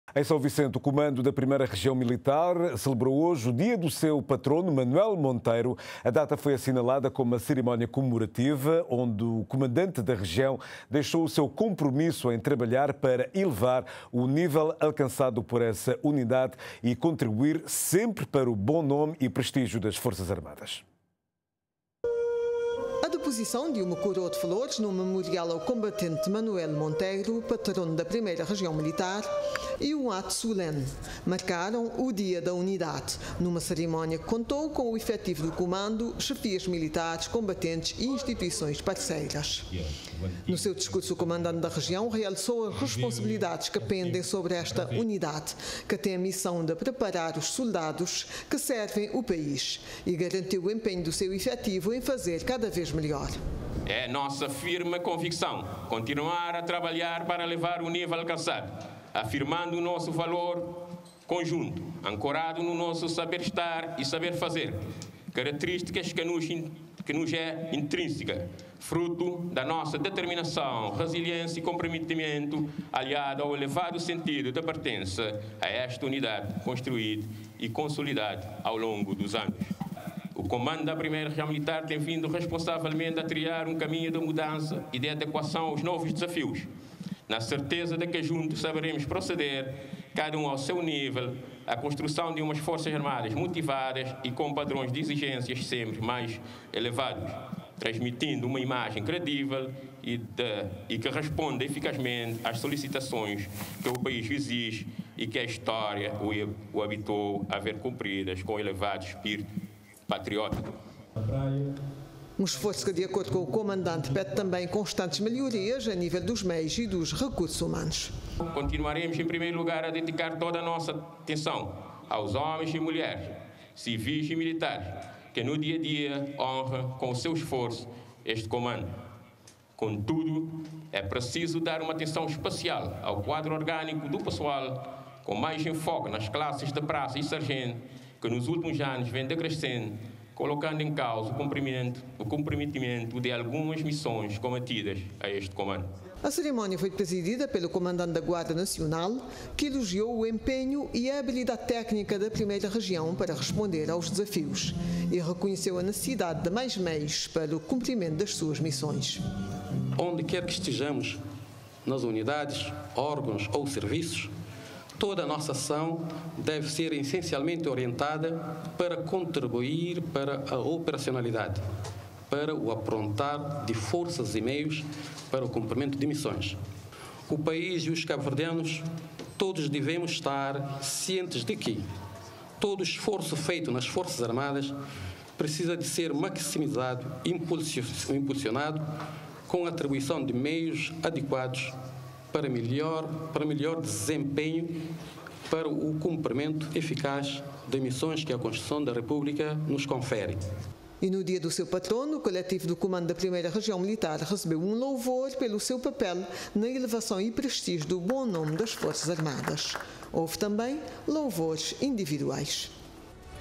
I Região Militar comemora o seu dia com o compromisso de continuar a prestigiar as Forças Armadas
Primeira Região Militar comemora o seu dia com o compromisso de continuar a prestigiar as Forças Armadas

RTC Online. Aqui, você está em Cabo Verde!